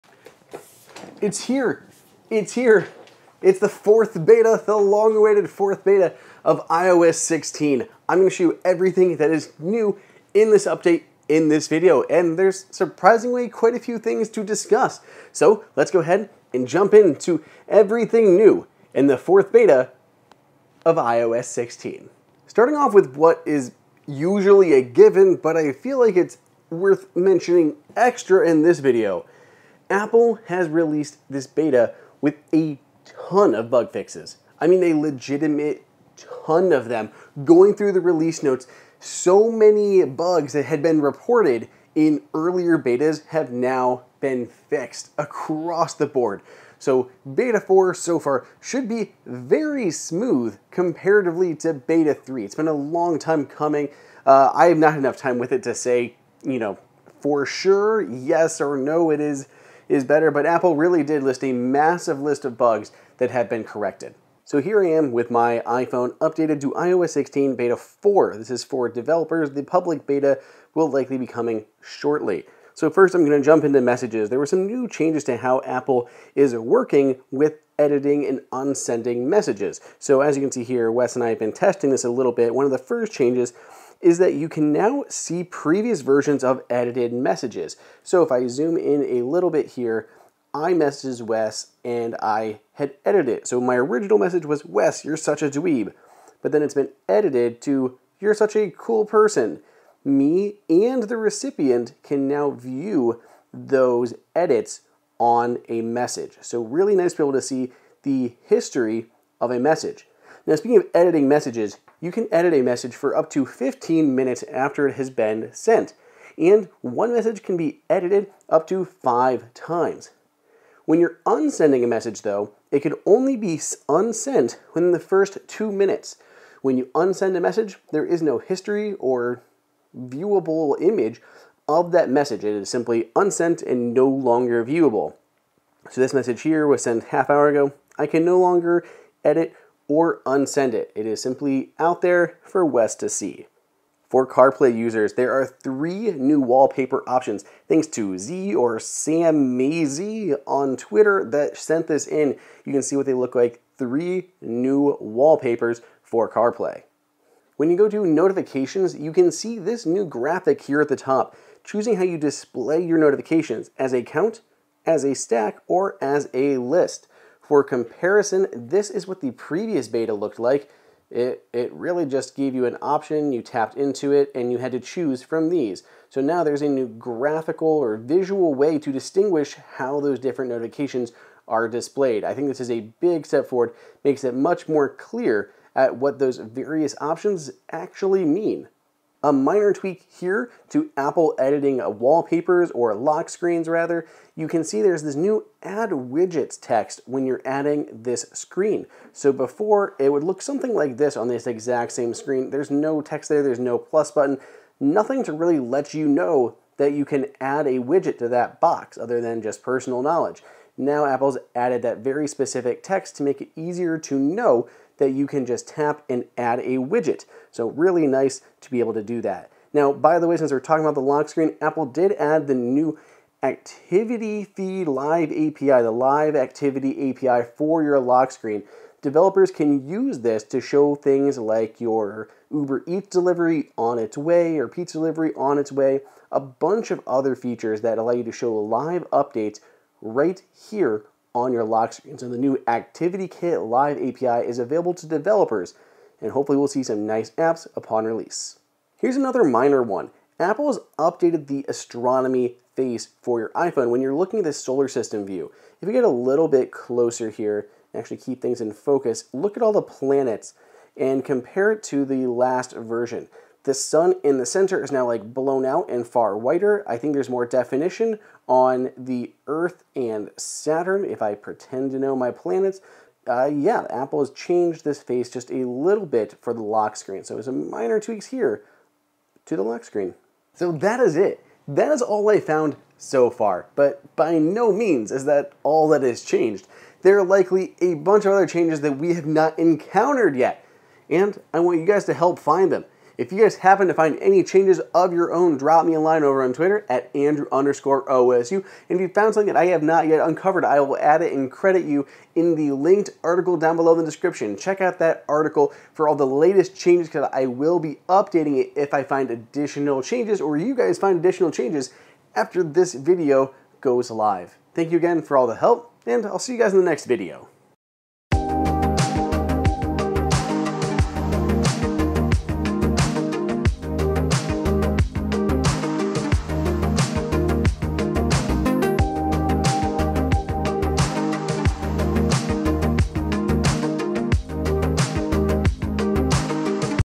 Everything NEW in iOS 16 Beta 4! Live Activities, Editing Messages, & More!
I've got the fourth developer beta of iOS 16 installed on my iPhone 13 Pro. I walk you through all the new changes and features that have arrived this time around. Have you spotted anything else new?

Chapters ➡
0:00 Intro
0:22 Bug Fixes
1:04 Messages
2:23 CarPlay wallpapers
2:37 Notification Settings
3:08 Photographic Styles
3:32 Lock Screen